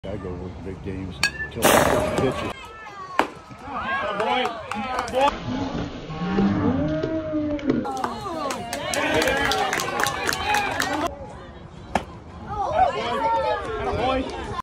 Is that a kid umpire?!?!
12U Game between the Yankees and the Royals with 8 year old Lathan The Kid Umpire behind the plate.